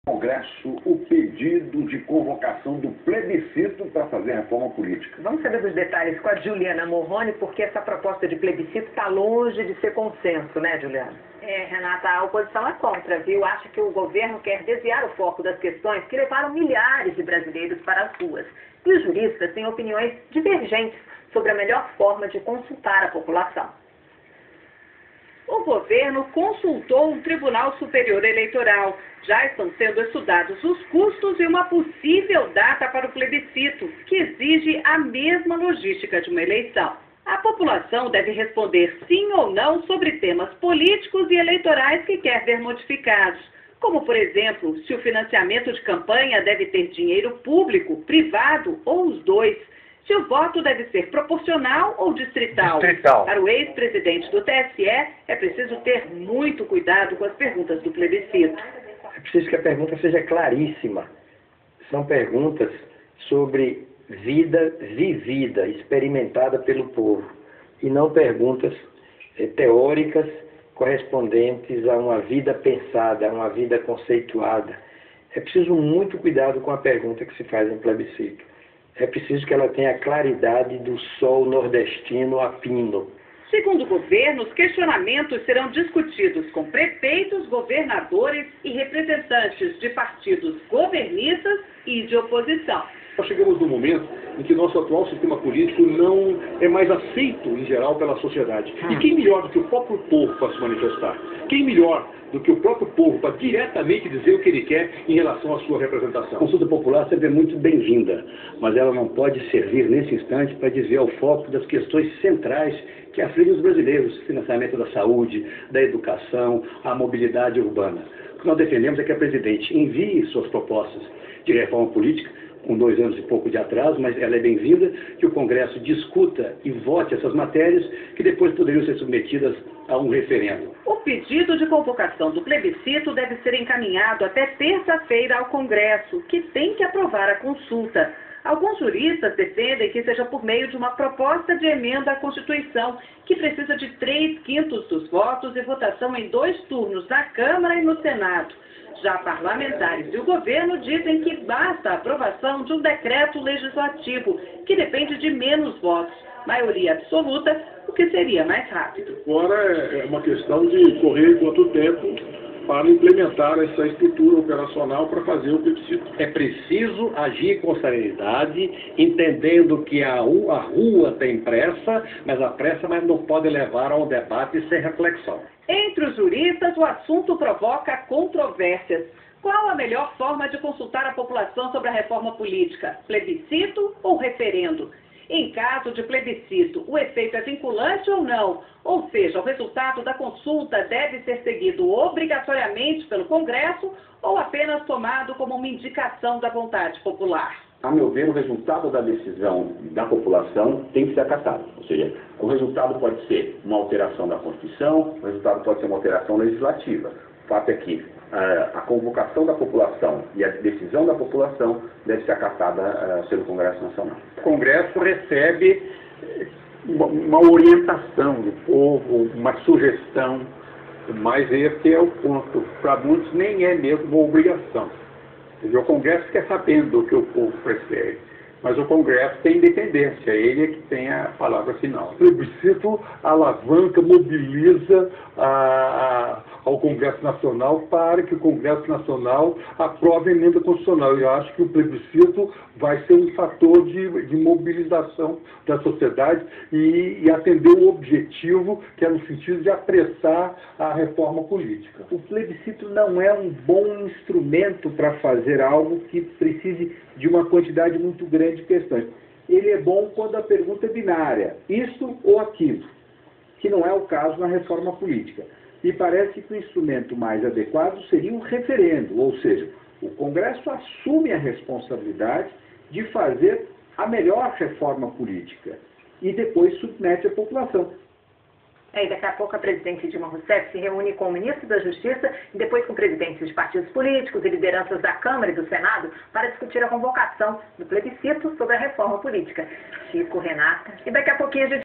CONSTITUIÇÃO FEDERAL ARTIGO 14
O voto DISTRITAL é a VERDADEIRA DEMOCRACIA  e é a forma de FAZER o nósso País se tornar verdadeiramente um PAÍS DESENVOLVIDO.